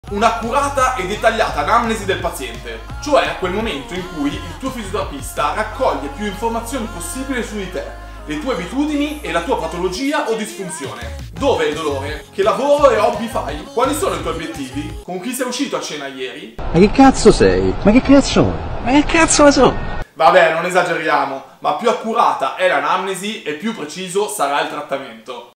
L'anamnesi in fisioterapia
In fisioterapia uno dei passaggi fondamentali per il successo della riabilitazione è l'anamnesi cioè quel momento in cui il tuo fisioterapista ti pone alcune domande su di te, la tua patologia, i tuoi obiettivi etc.
L'anamnesi è importante perchè permette al fisioterapista di capire la problematica e orientare tutto il resto del trattamento in base alle informazioni che sono venute fuori in fase di colloquio con il paziente.
Non sottovalutare l'importanza dell'anamnesi e buon trattamento fisioterapico.

💪🏽 Fisioterapia Muggiò via Sondrio 49